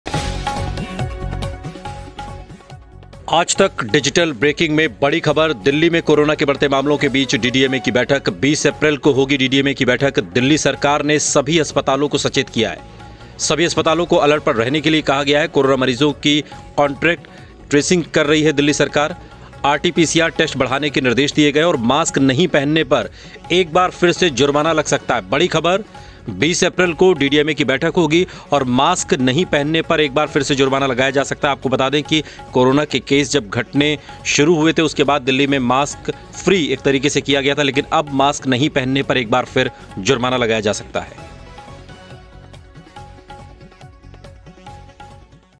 Delhi में कोरोना के मामलों ने पकड़ी रफ्तार, Mask पहनना हो सकता है अनिवार्य। Breaking News
दिल्ली में कोविड-19 के मामलों में तेजी को देखते हुए दिल्ली आपदा प्रबंधन प्राधिकरण (DDMA) शहर में कोरोना वायरस के प्रसार को रोकने के उपायों पर विचार करने के लिए 20 अप्रैल को एक बैठक करेगा. इसी बीच दिल्ली सरकार ने सभी अस्पतालों को अलर्ट कर दिया है.